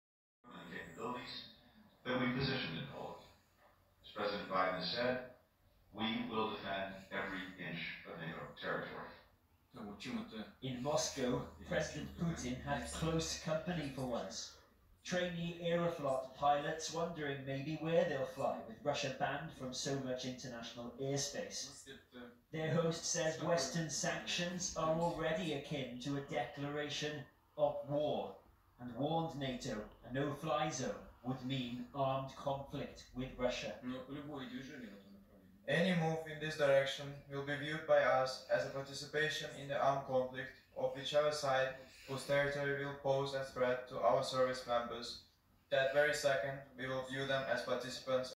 In Moscow Precedent Putin had close company for once, trainee Aeroflot pilots.
In Moscow Precedent Putin had close company for once, trainee Aeroflot pilots wondering maybe where they fly . . . Quite militarily, politically intimidating wouldn't you agree